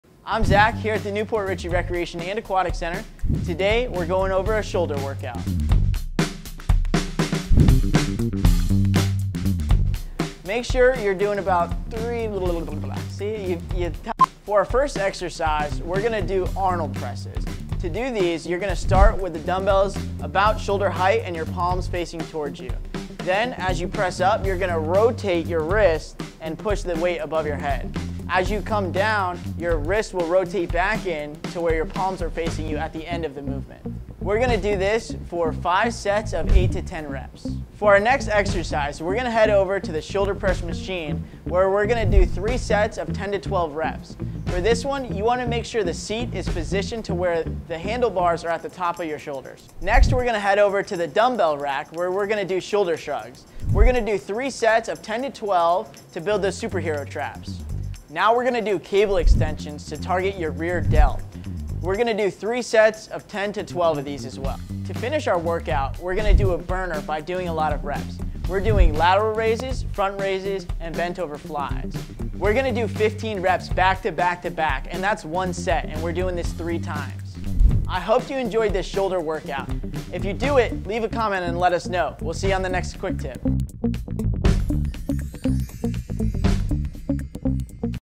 Shoulder Workout
Here is an example of a full shoulder workout! Let us know if you give it a try.

-Arnold Press 5 x 8-10
-Shoulder Press Machine 3 x 10-12
-Shoulder Shrugs 3 x 10-12
-Cable Extensions 3 x 10-12
-Shoulder Burner 3 x
15 lateral raises + 15 Front Raises + 15 Bentover Flies